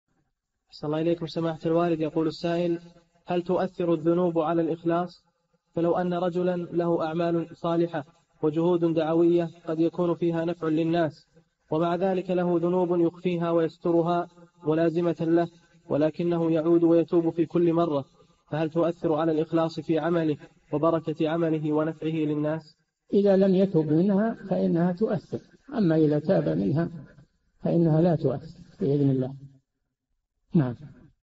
[1835- 4717] هل تؤثر الذنوب على الإخلاص وبركة الأعمال الدعوية؟ - الشيخ صالح الفوزان
[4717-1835] هل تؤثر الذنوب على الإخلاص فلو أن رجلا له أعمال صالحة وجهود دعوية قد يكون فيها نفع للناس ومع ذلك له ذنوب يخفيها ويسترها ملازمة له ولكنه يعود ويتوب في كل مرة، فهل تؤثر على الإخلاص في عمله وبركة عمله ونفعه للناس؟
الجَوَابُ: إذا لم يتب منها فإنها تؤثر، أما إذا تاب منها فإنها لا تؤثر بإذن الله.
السيرة الذاتية :
للاشتراك في قناة الشيخ صالح الفوزان
لمشاهدة حلقات فتاوى على الهواء كاملة

للاستفسار والإقتراحات والدعم
رئيس الفريق
لمشروع كبار العلماء